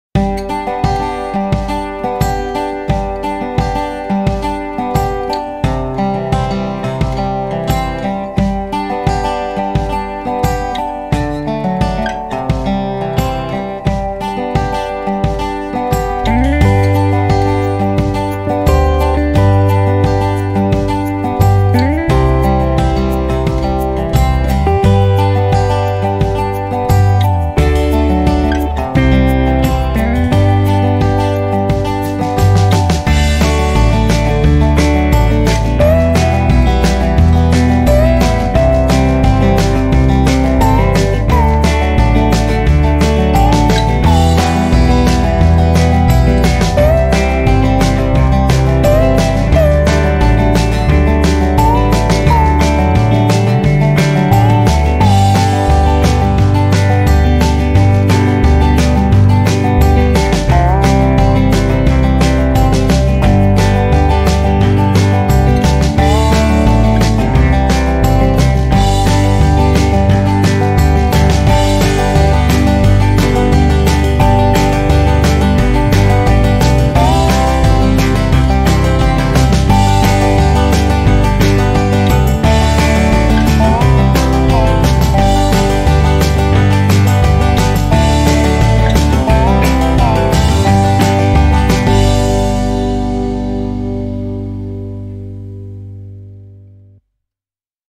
Views of my 2019 Summer
A montage of all the panoramic videos I took whilst traveling around the Western United States of America. I got to see Vail, Colorado River, Moab, Grand Canyon, Sedona, Yosemite and San Francisco. I realised halfway through my trip that I was always panning left to right when taking scenic video shots with my GoPro hero 7 black so I decided to put them all into a blended panoramic video. If I do a trip like this again I might try to make one of these again but get a more consistent panning speed